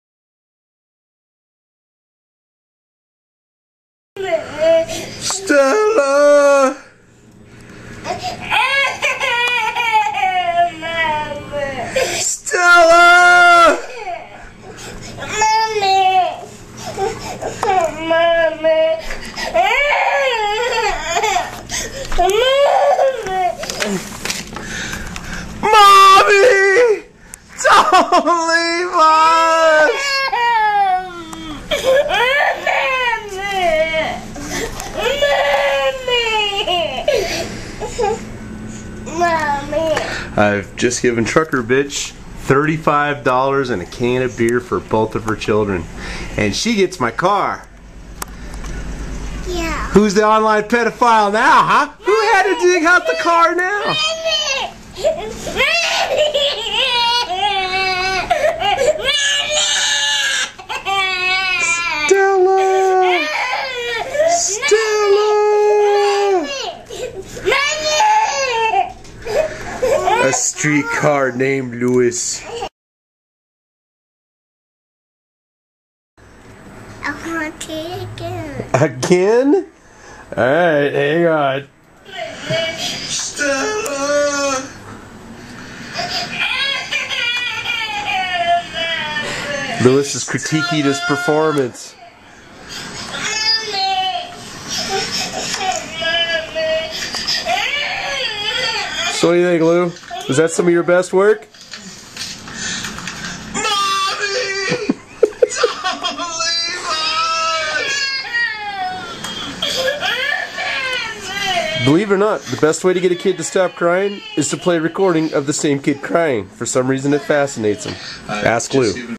cowkiller streetcar named lou
Grandma is DOWN..mommy had to rush to the hospital.
and cowkiller is stuck with
"Whiney McBoohoo"